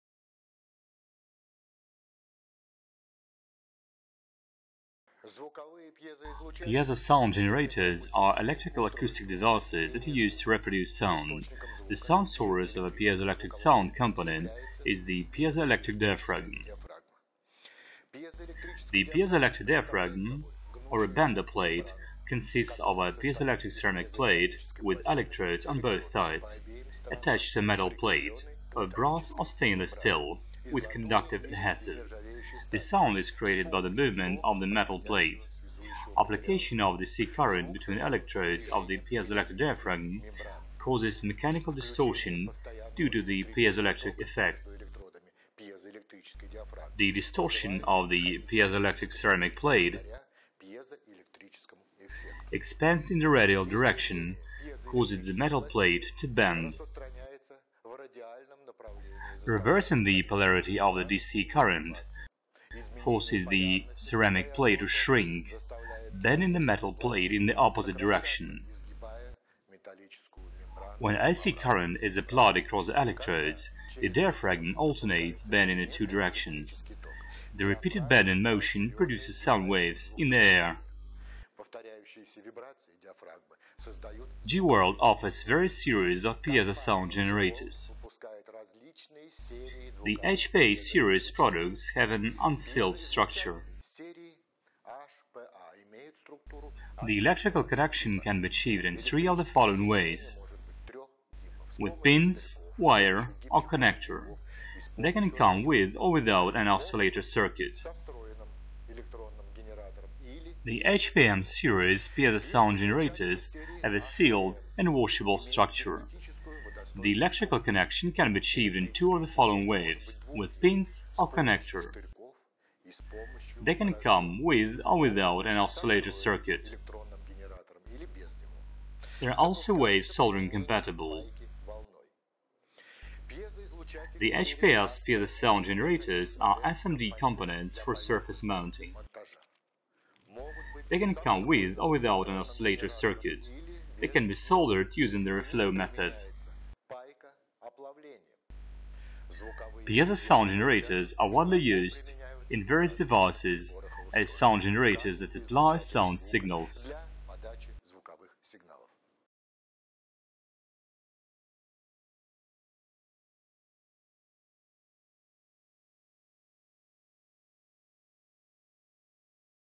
Jl World's Piezo Sound Generators
Jl World's Piezo Sound Generators                Piezo sound generators are electrical acoustic devices that are used to reproduce sound. The sound source of a piezoelectric sound component is the piezoelectric diaphragm.         The piezoelectric diaphragm or a bender plate consists of a piezoelectric ceramic plate, with electrodes on both sides, attached to a metal plate (of brass or stainless steel) with conductive adhesive.        The sound is created by the movement of the metal plate. Application of D.C. current between electrodes of the piezoelectric diaphragm causes mechanical distortion due to the piezoelectric effect.         The distortion of the piezoelectric ceramic plate expands in the radial direction causing the metal plate to bend. Reversing the polarity of the D.C. current forces the ceramic plate to shrink, bending the metal plate in the opposite direction. When A.C. current is applied across the electrodes, the diaphragm alternates bending in the two directions. The repeated bending motion produces sound waves in the air.        Jl World offers various series of piezo sound generators.         The HPA series products have an unsealed structure. The electrical connection can be achieved in three of the following ways: with pins, wire or connector. They can come with or without an oscillator circuit.        The HPM series piezo sound generators have a sealed and washable structure. The electrical connection can be achieved in two of the following ways: with pins or connector. They can come with or without an oscillator circuit.        They are also wave soldering compatible.        The HPS piezo sound generators are SMD components for surface mounting. They can come with or without an oscillator circuit. They can be soldered using the reflow method.        Piezo sound generators are widely used in various devices as sound generators that supply sound signals.